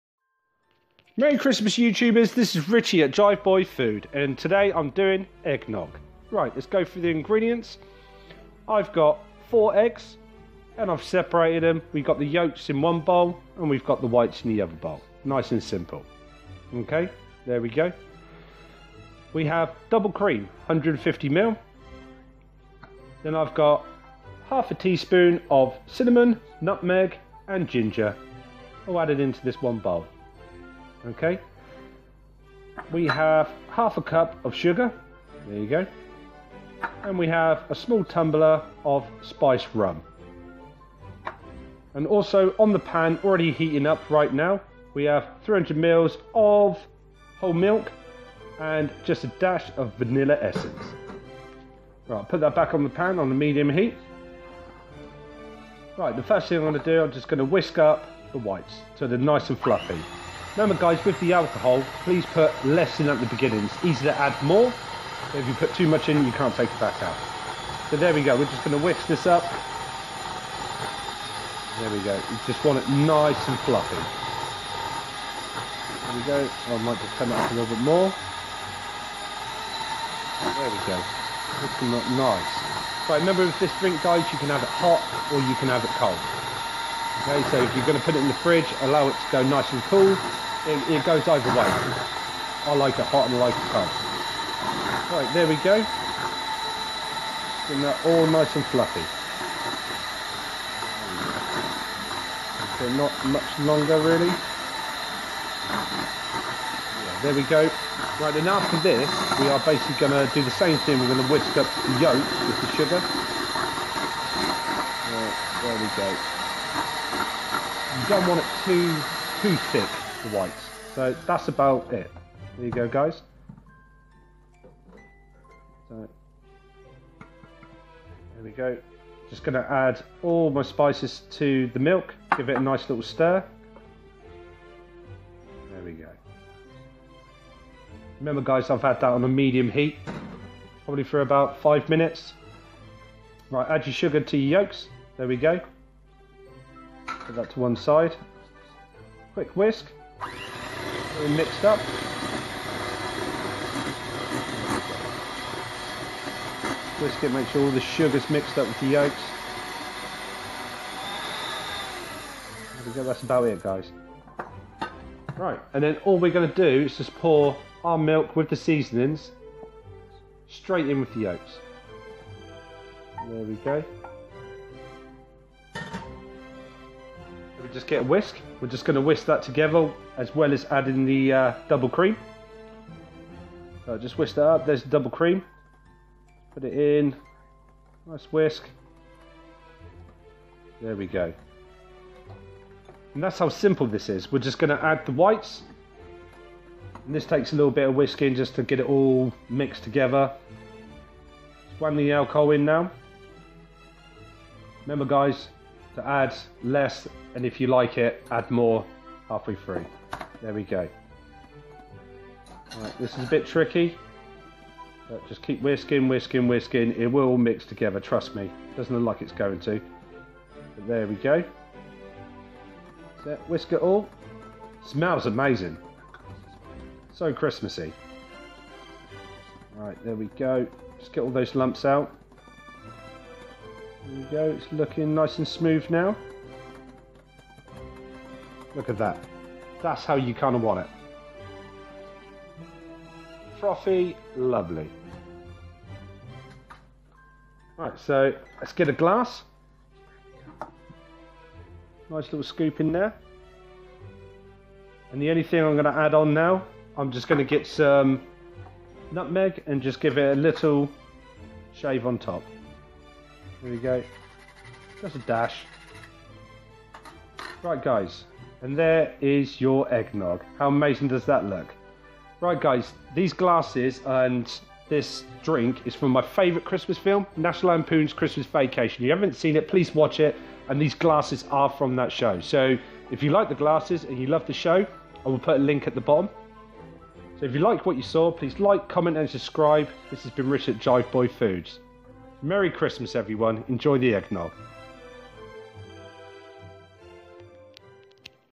How to make Eggnog
Learn how to make Homemade Eggnog!  many more video recipes! I hope you enjoy this easy Classic Christmas Eggnog recipe.